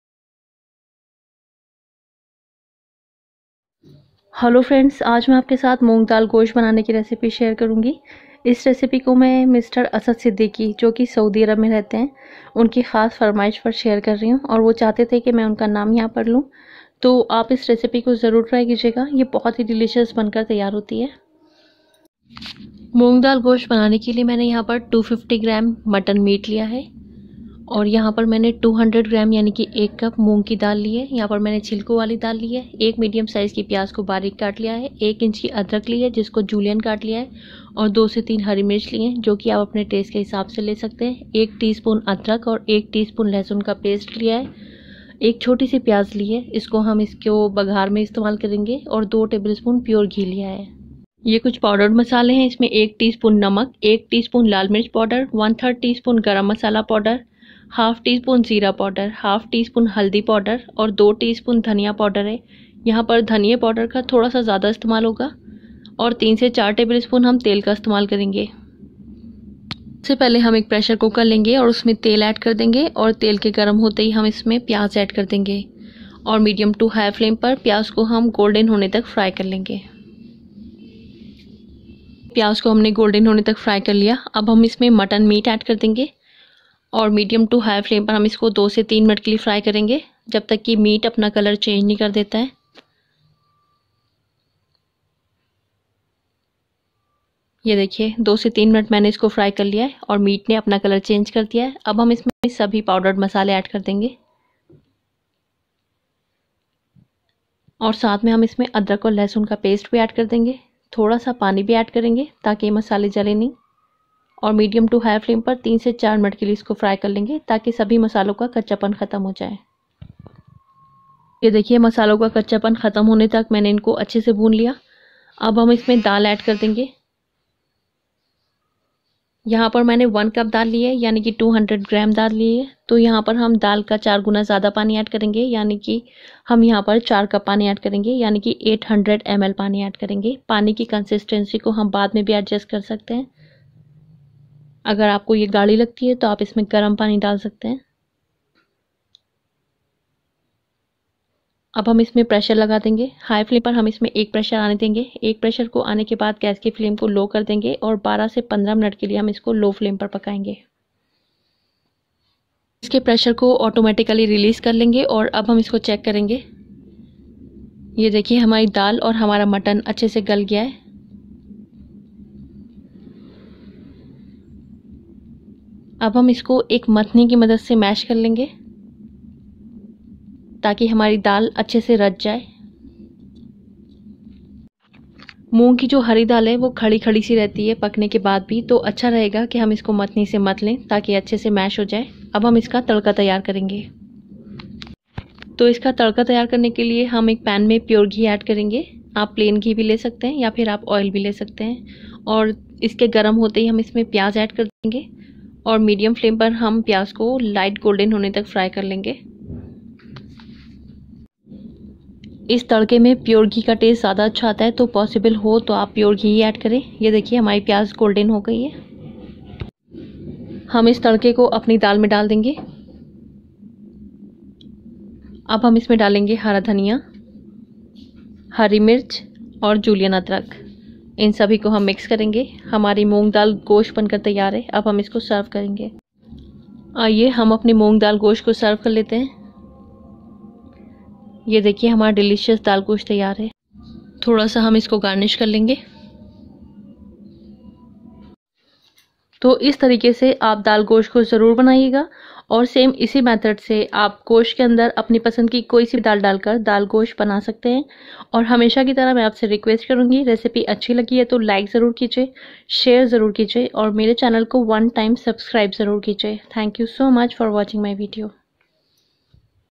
Dal Gosht Recipe/ Moong Daal Gosht/ दाल गोश्त रेसिपी
Delicious Daal Gosht Recipe..
Ingredients:
250 g mutton
200 g moong daal( 1 cup)
1 medium size onion
1 inch ginger Julian
2-3 green chillies
1+1 tsp ginger garlic paste
1 tsp salt
1 tsp red chili powder
1/2tsp turmeric powder
1/2 tsp cumin powder
1/3 tsp garam powder
2 tsp coriander powder
1-2 tbsp pure ghee
1 small onion
3-4 tbsp oil